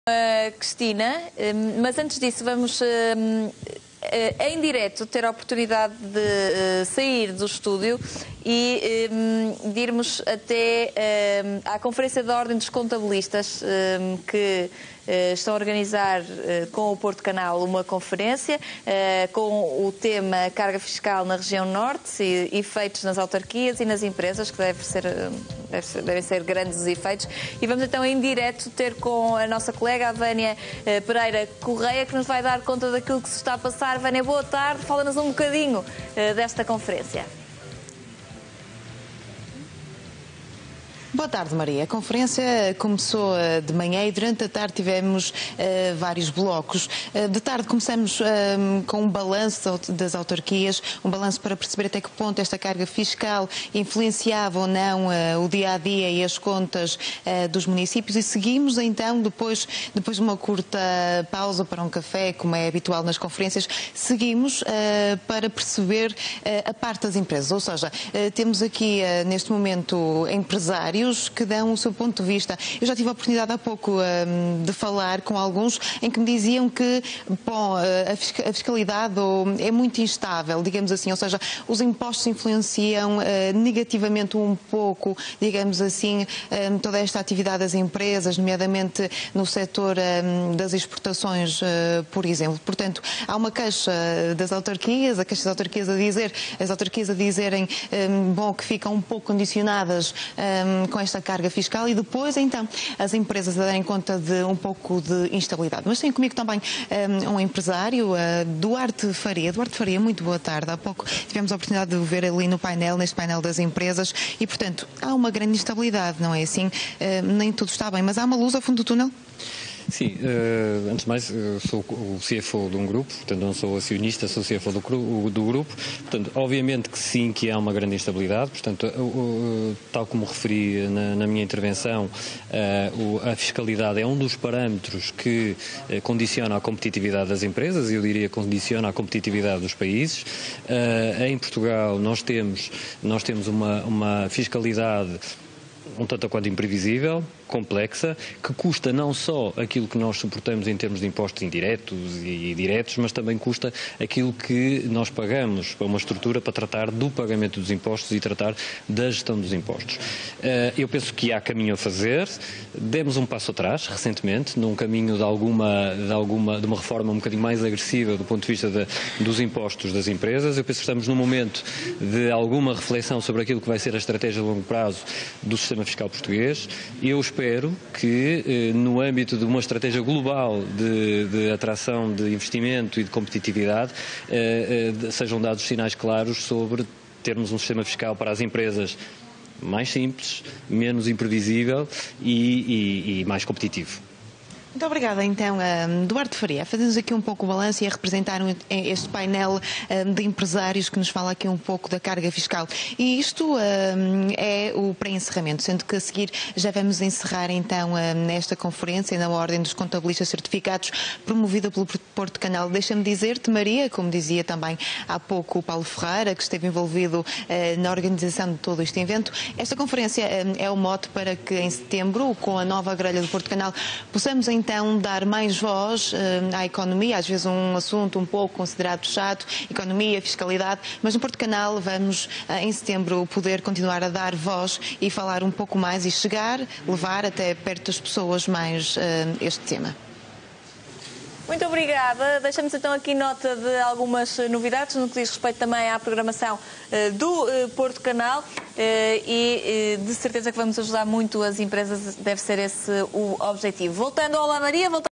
Porto Canal - Conferência A Carga Fiscal na Região Norte - V
Empresários do norte debatem nível da carga fiscal aplicada à região em conferência organizada pelo Porto Canal e a Ordem. Declarações de Duarte Faria, administrador da Frulact